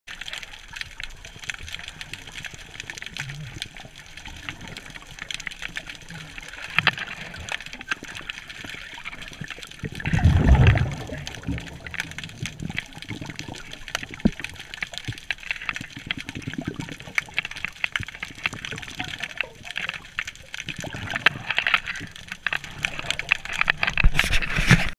8/23/14
lovers point is a great spot for introducing free diving/ to friends